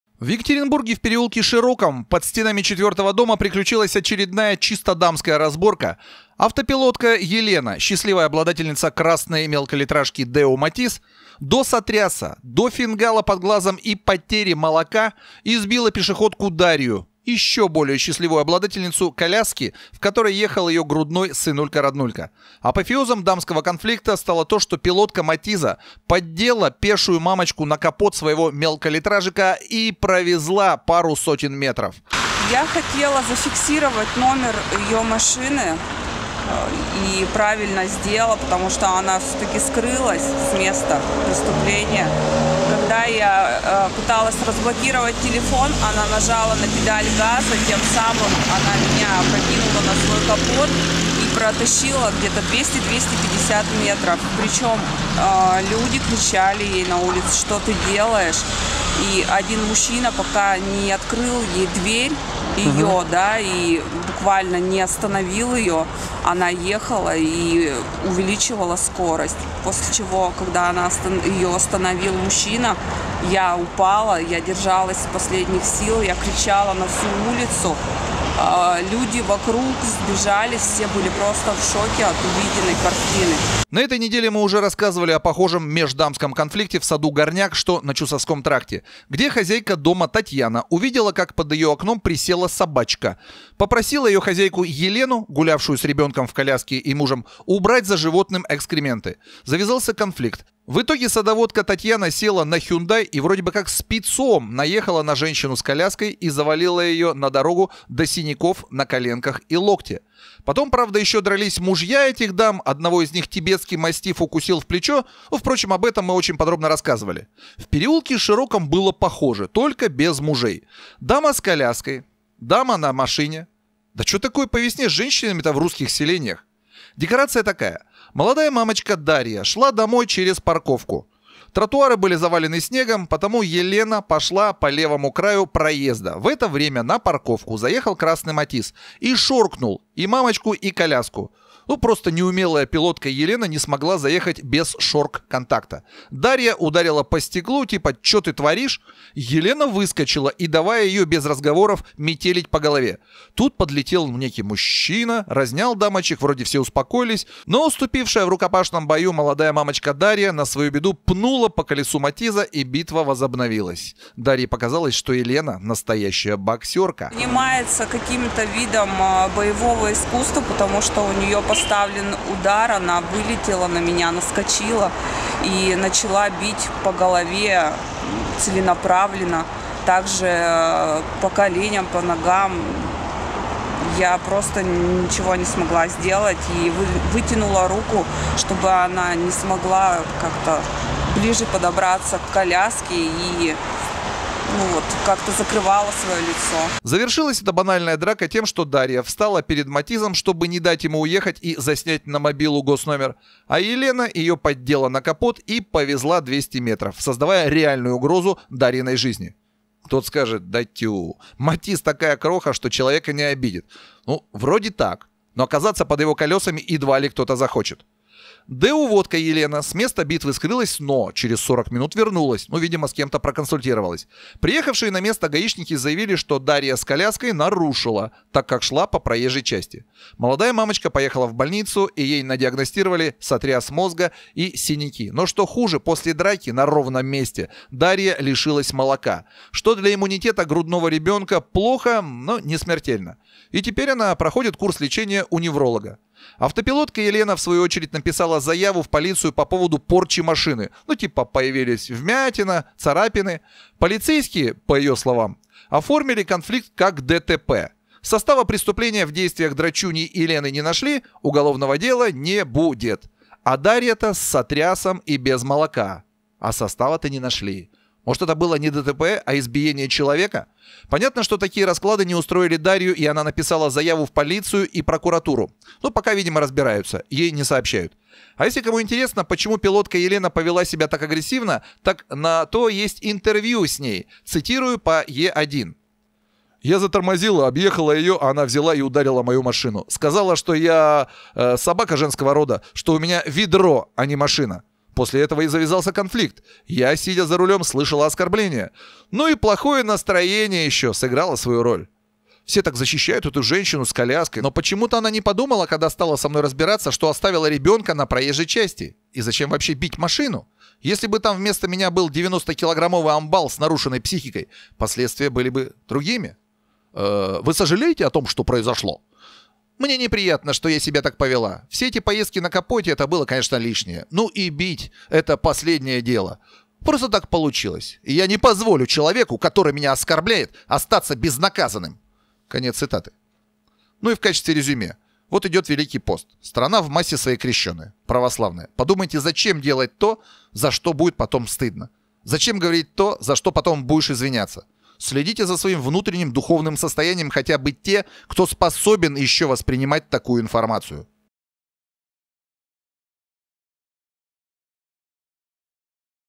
Пилотка "Матиза" метелит мамочку с коляской. Real video
Мамочка сотрясла мозг и лишилась молока. Где уголовное дело?